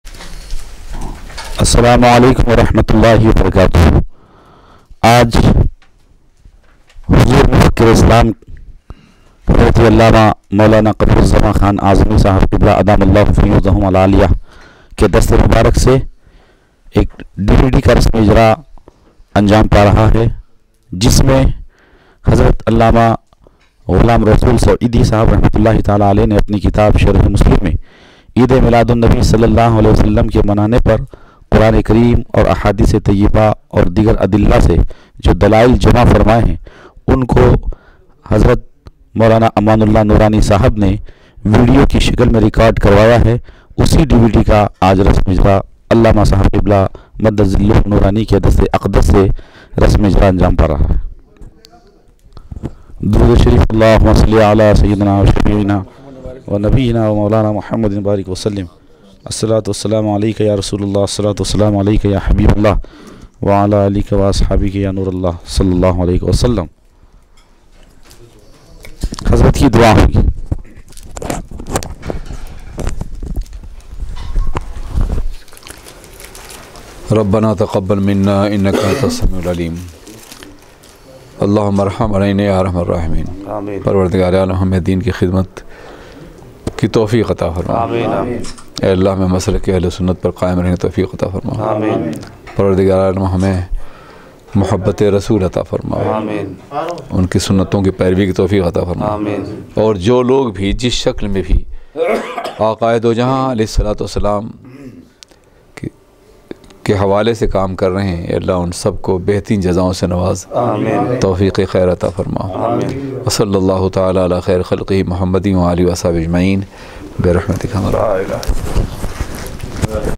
DVD Launch By Allama Qamaruzzaman Khan Azmi London, Milad Un Nabi Quran Hadeeth Aur Aqwale Ulema Se
DVD Launch By Allama Qamaruzzaman Khan Azmi London, Milad Un Nabi, Sallallaahu Alaihi Wa Sallam Quran Hadeeth Aur Qadeem Ulema Ke Aqwal Ki Roshni Mein, From The Book Of "Sharah Muslim" Allama Ghulam Rasool Sayeedi Alaihirrahma, Is Bayan Ki Link-